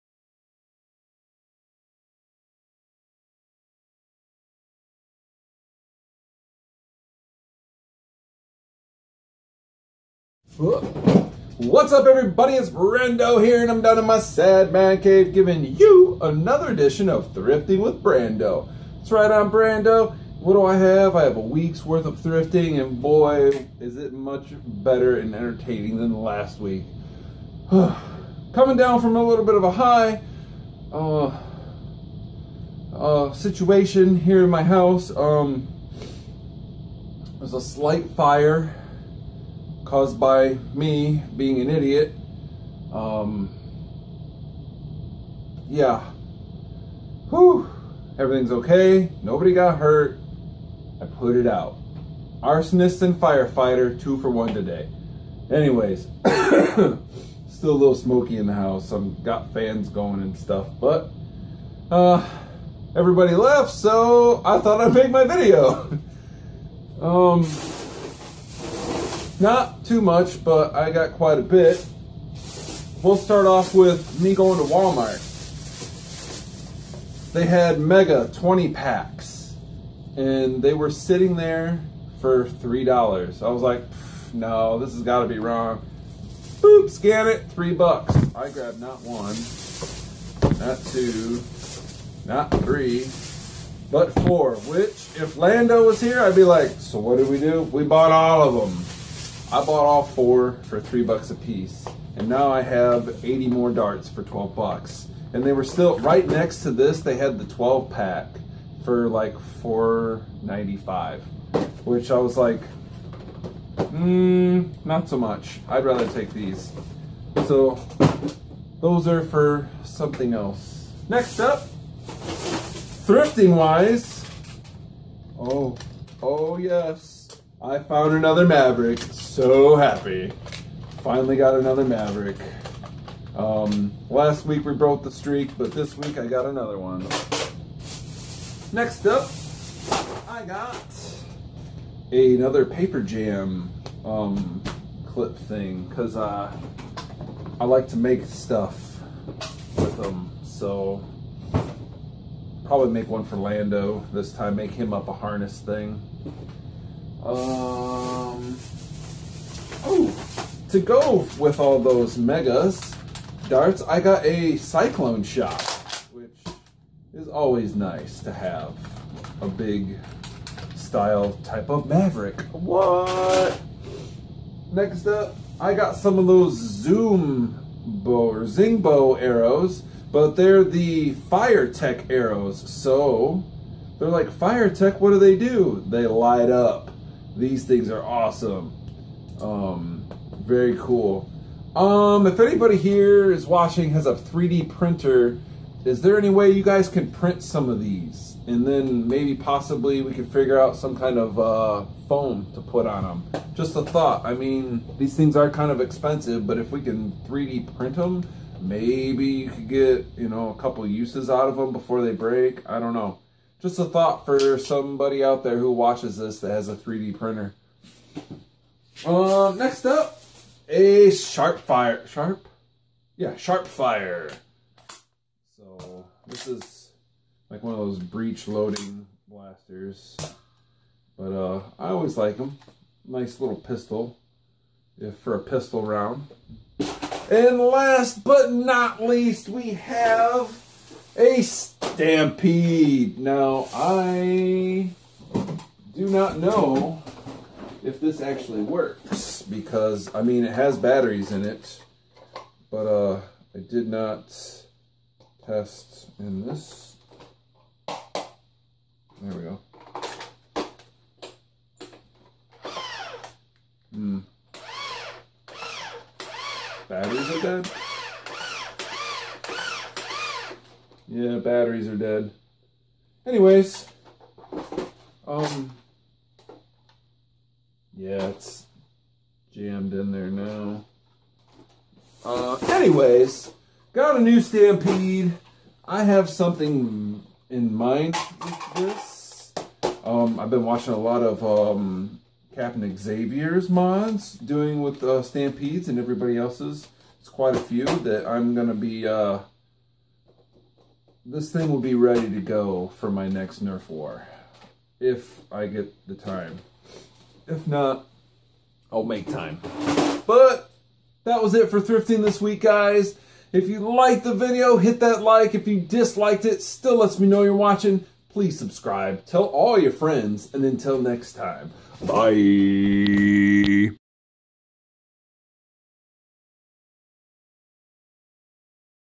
NERF THRIFTING EPISODE: 42
Had a good week. No the fire wasn't bad. Just a little one. Nothing ruined.